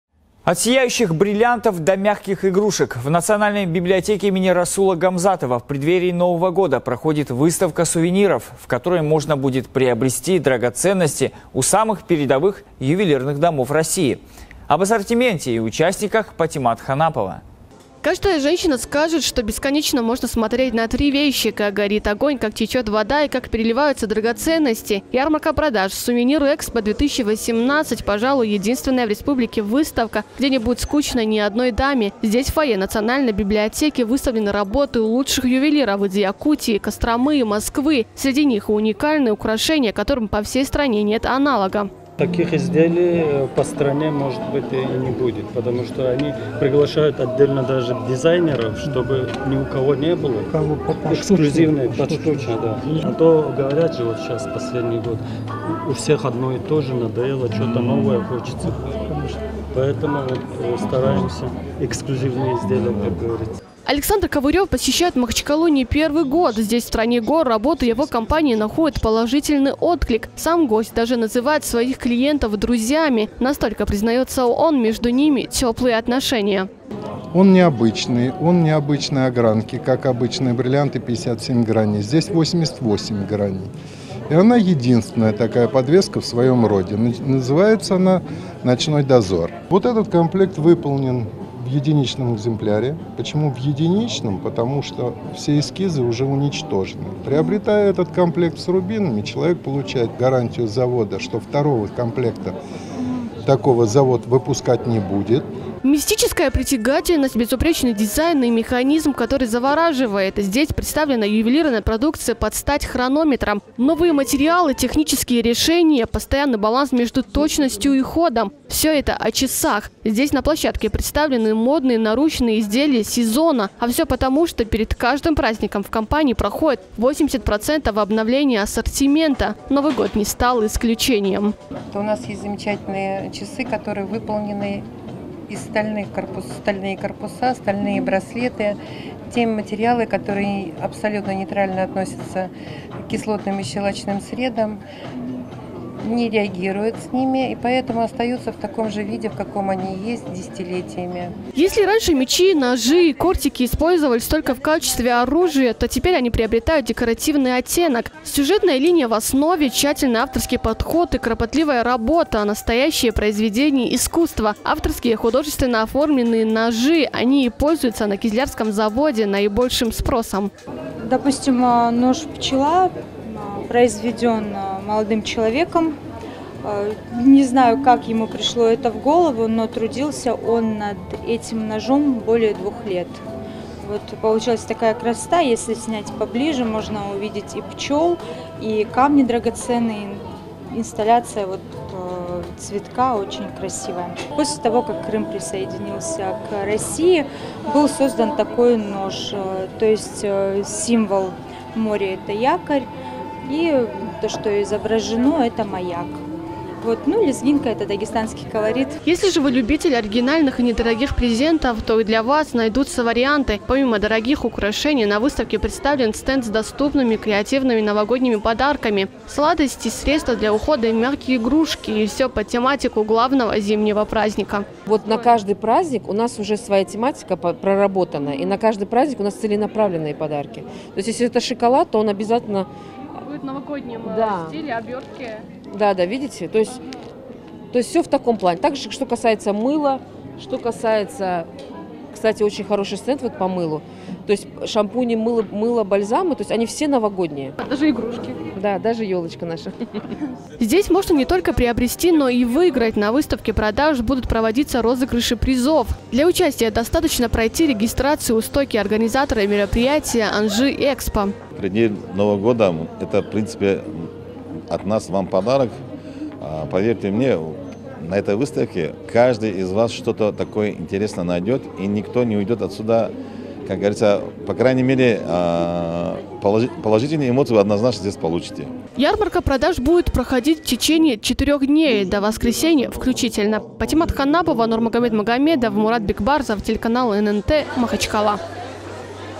Розыгрыш ювелирных изделий и подарков в Махачкале
От сияющих бриллиантов до мягких игрушек. В национальной библиотеке им. Расула Гамзатова в преддверии Нового года проходит выставка сувениров, в которой можно будет приобрести драгоценности у самых передовых ювелирных домов России.